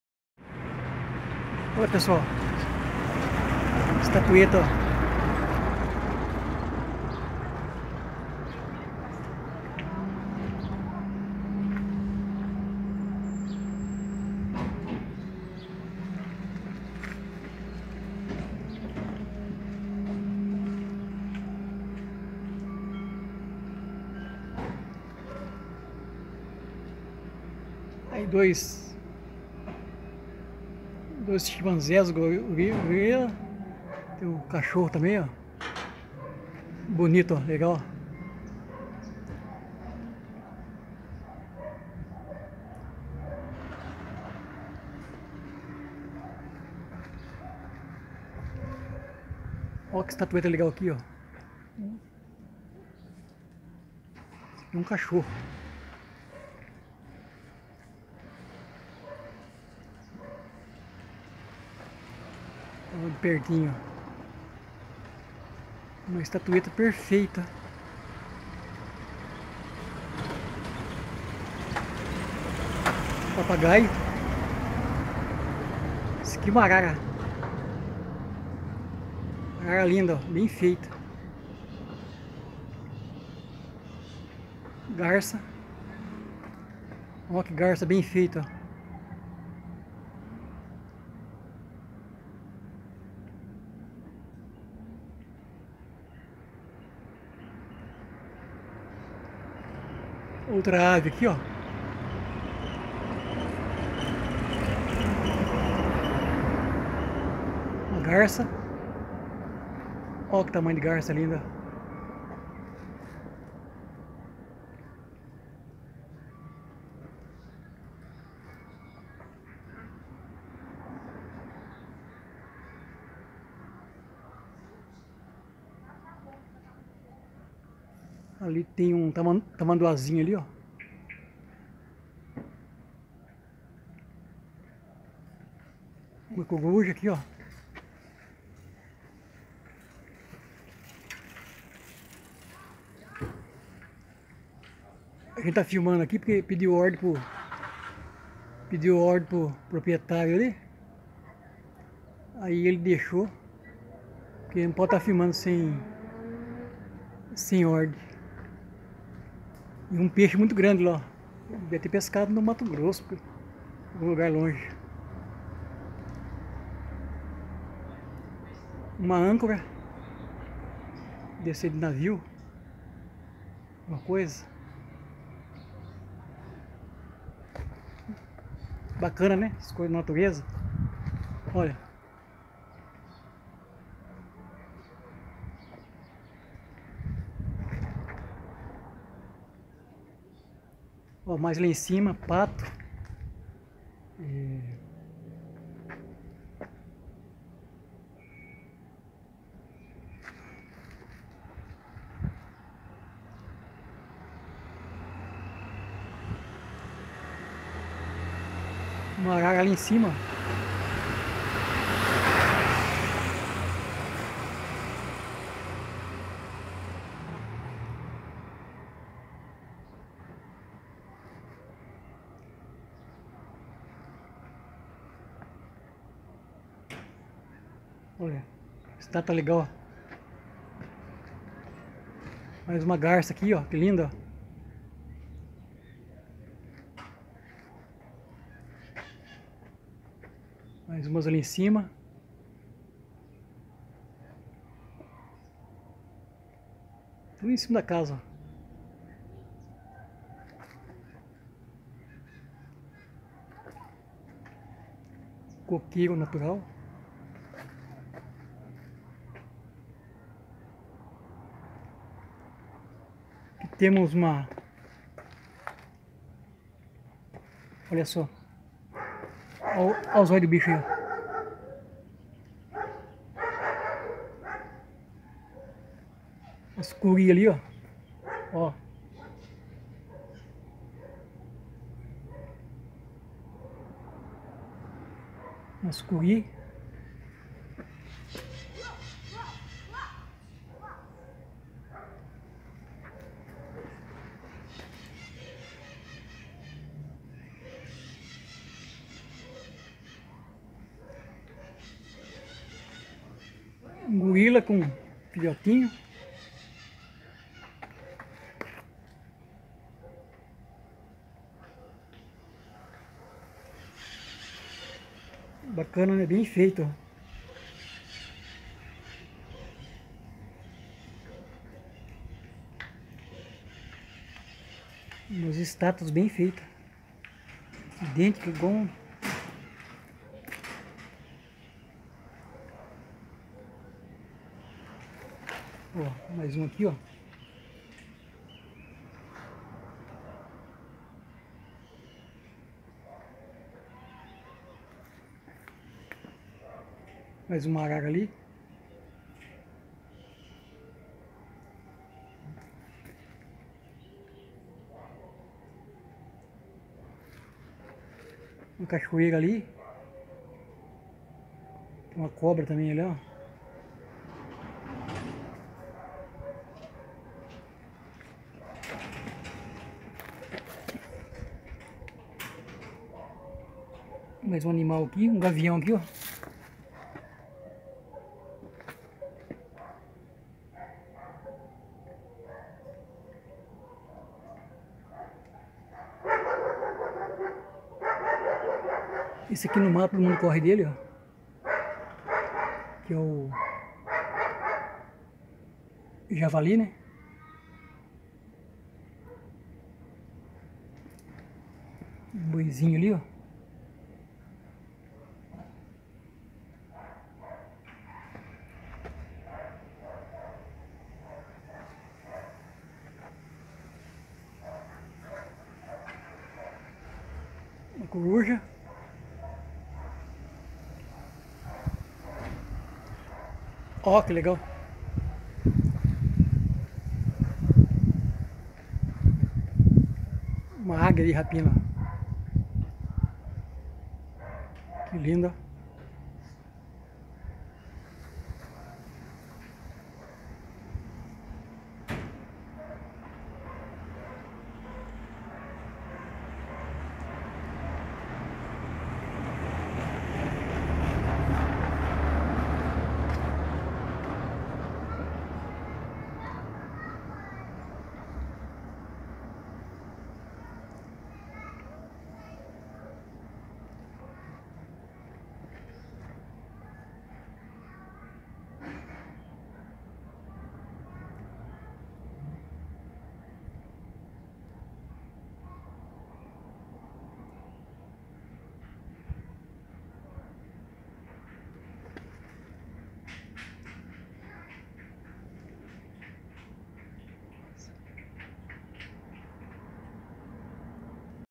Estatuetas De Aves e Animais Exótico
São  Lindas  perfeita, o   dono  desta  estatuas   e   colecionador   também    serve  como    decoração  em   sua     residencia,    Gostou  se   escreva    , não     custa  nada  ,  obrigado